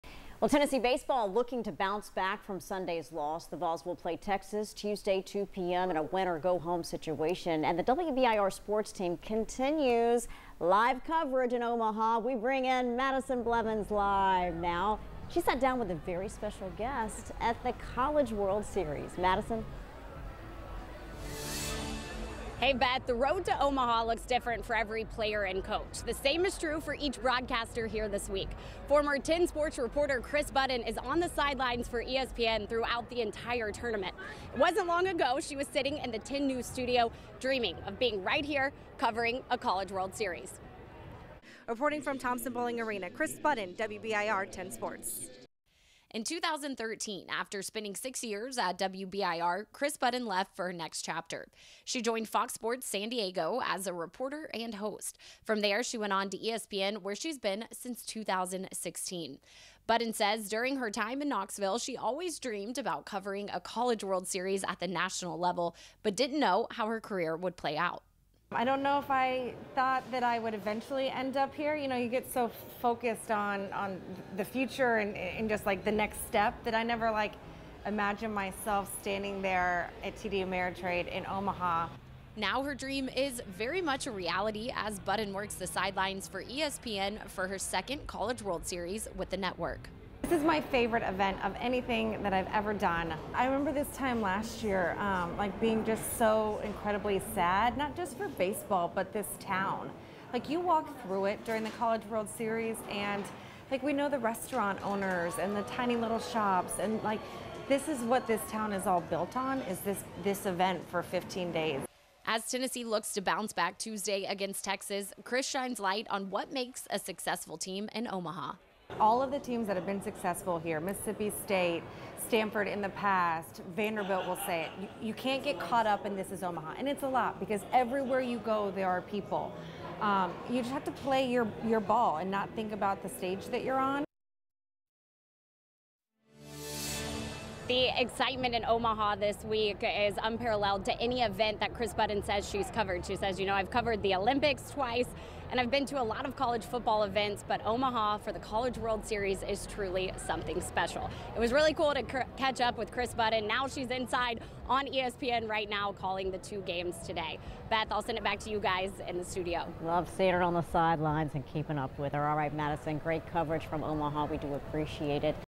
Former WBIR 10Sports reporter achieves dream of covering College World Series in Omaha
Kris Budden is on the sidelines for ESPN throughout the tournament.